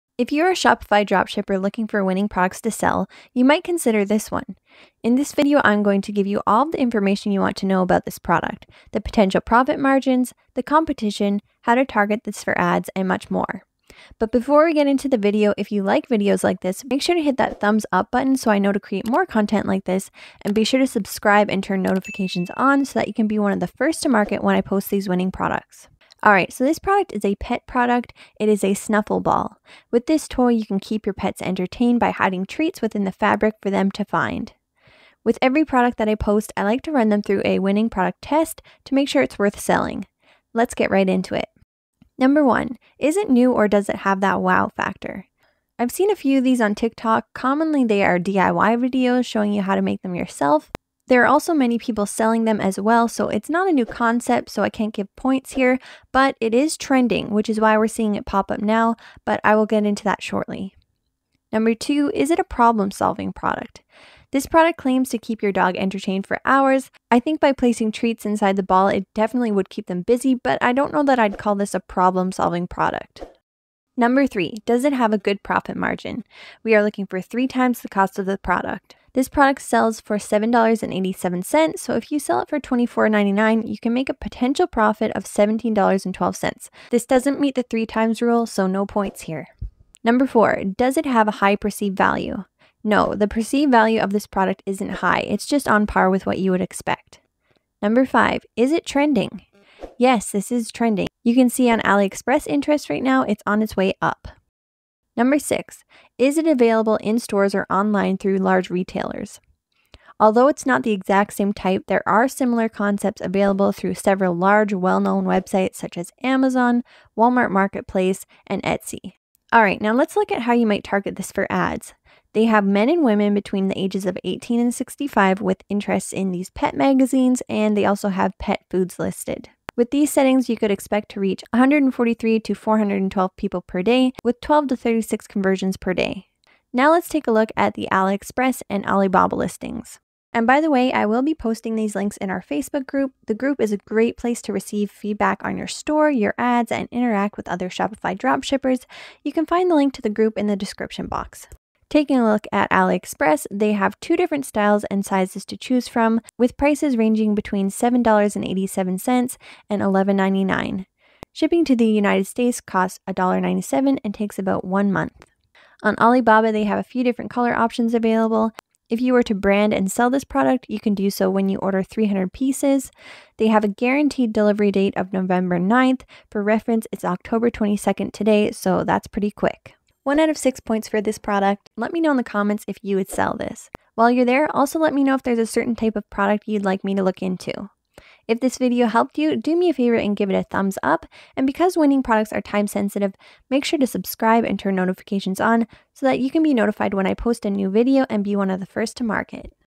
Winning Product Trending NOW!! Snuffle Ball!
If you are a Shopify dropshipper looking for winning products to sell, this video is for you! In this video the Ecom Geeks do the dropshipping product research for you and show you a Winning Product that you can start selling in your Shopify dropshipping store today.